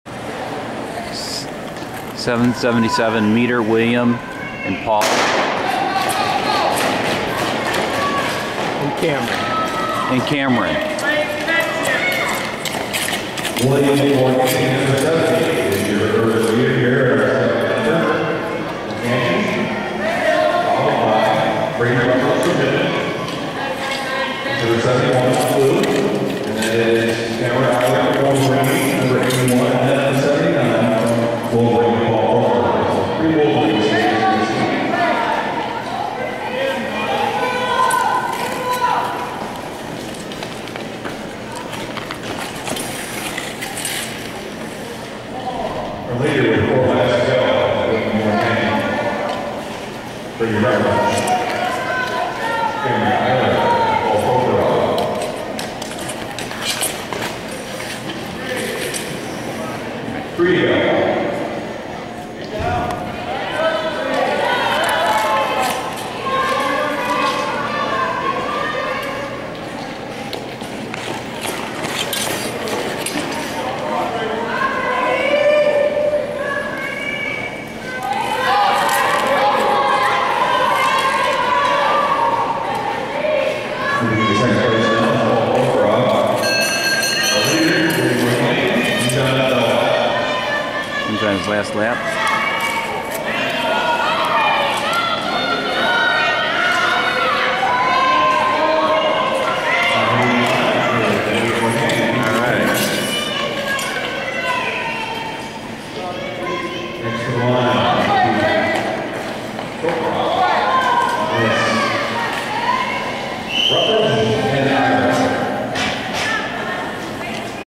Dec 6 and 7 2014 Midland Youth and Master Festival 041
777 meter this was my first year of competition in 4 years of competing  that I finally brought home  bronze  other races will be brought up  eventually by the end of that season got three medals in three diffreant competitions I look and skate much better than the videos I'm uploading 2015/16 these videos are tribute to my skating coach Dave Rondot thanks Dave